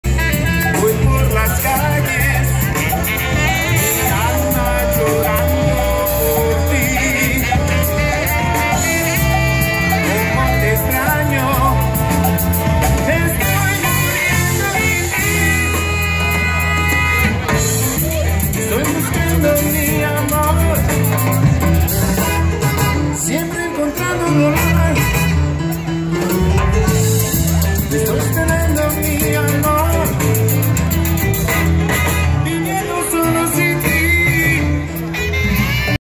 Pleasanton first Wednesday street fair, may 2016
Great music by Ruckatan